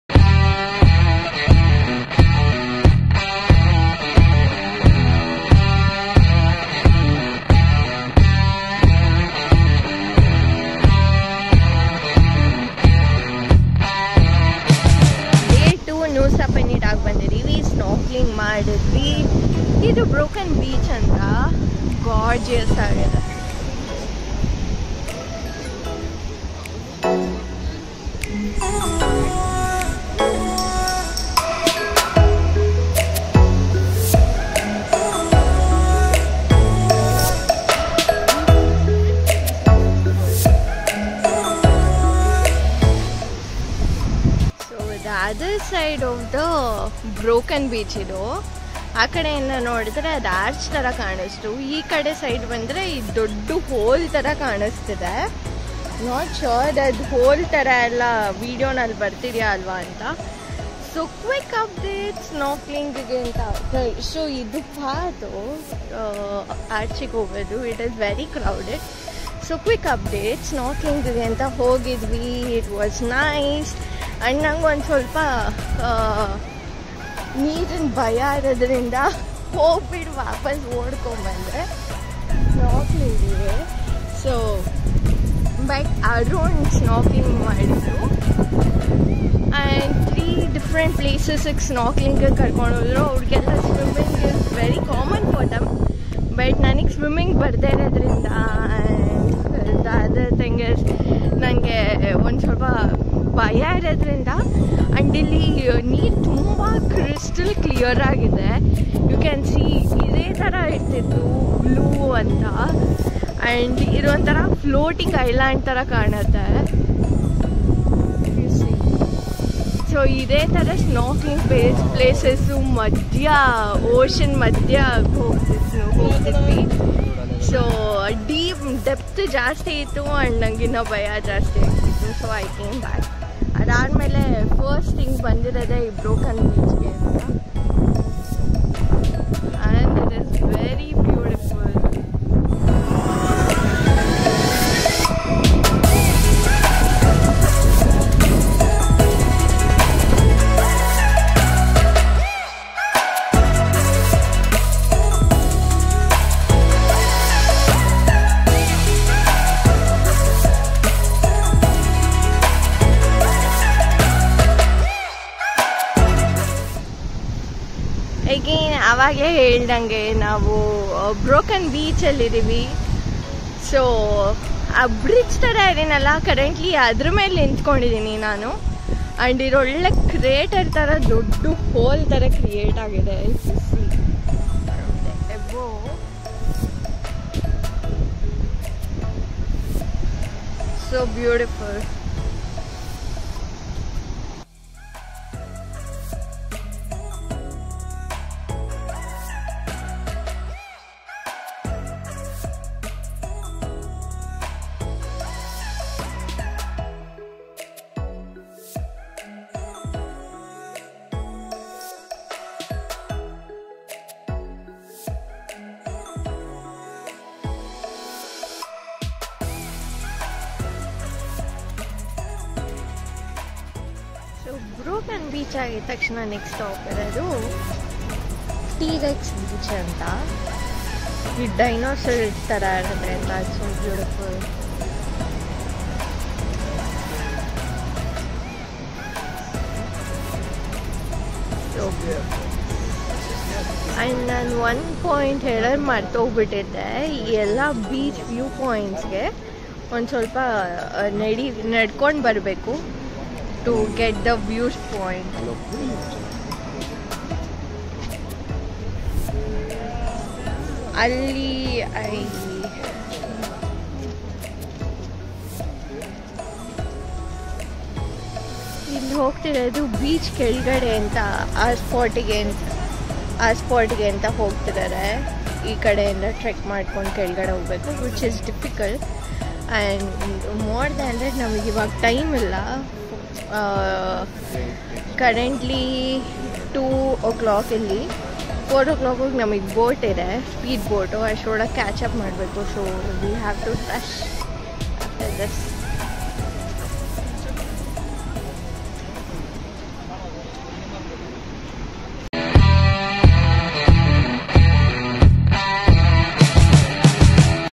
Broken Beach and Trex beach | Vlog 03 | Ep45
Deep Blue ocean and a natural arch structure mesmerizing scenic.